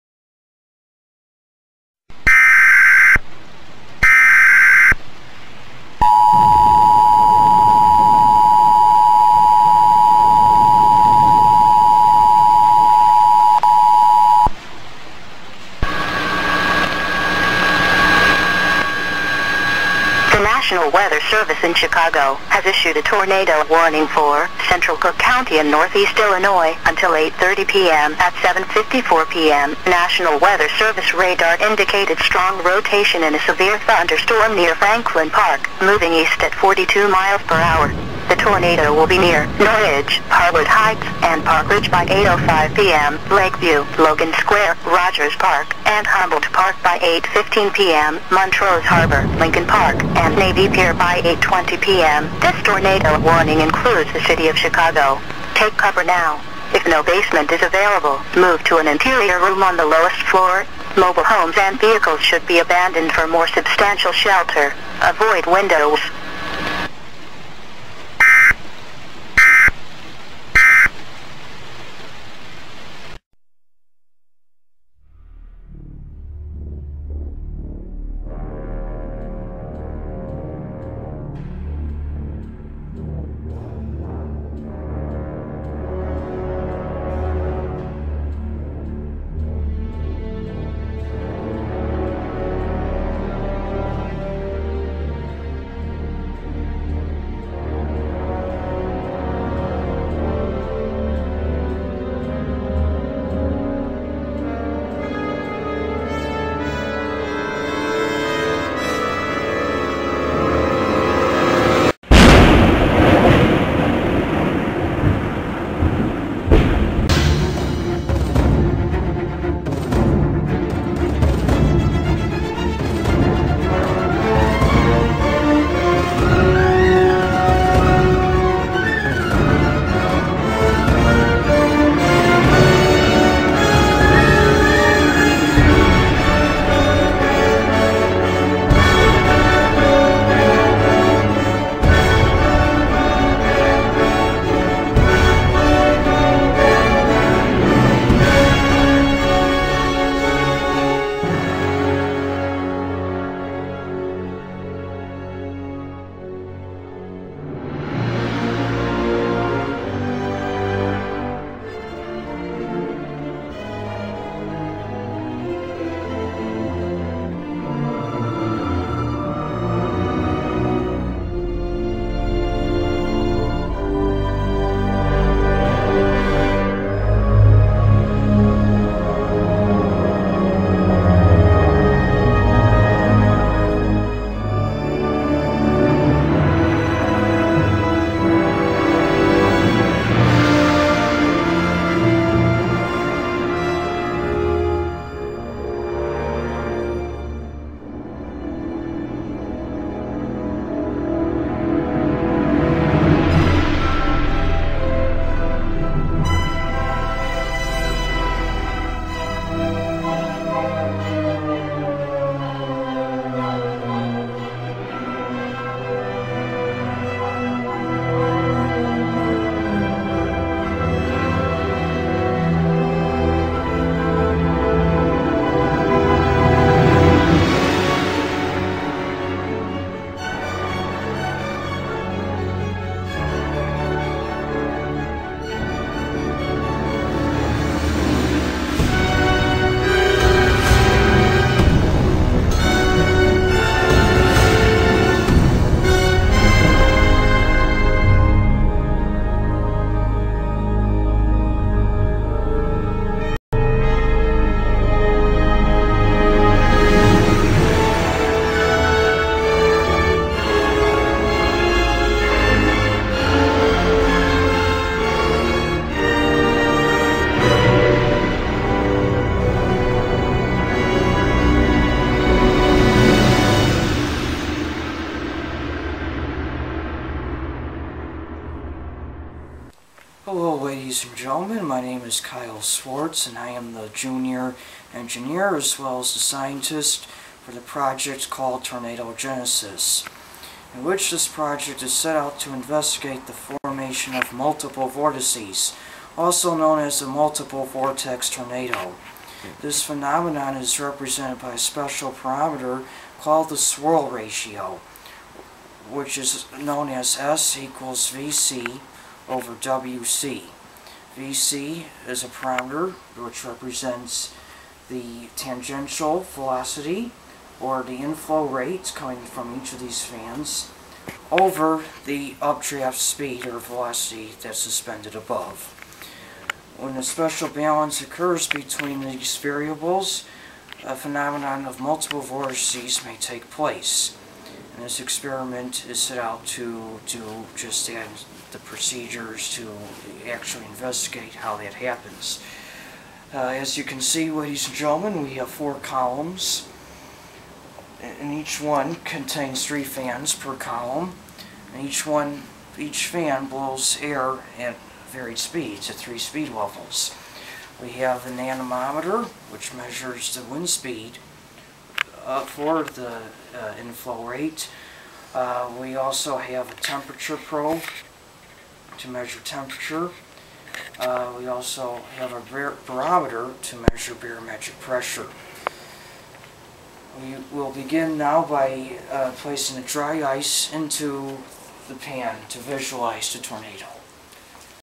Project Tornado Genesis: Multiple Vortices Part 1/4
Project 'Tornado Genesis: Multiple Vortices' investigates the conditions that favor the formation of multiple vortices. I have designed and constructed a large-scale tornado machine generator for the operation of a 7.5 foot tall tornado. This machine provides all the necessary variables to conduct my experiments and its design allows for the application of an important parameter called the swirl ratio (important for determining vortex structure and ultimately observing multiple vortices upon reaching a critical value). Continue to Part 2/4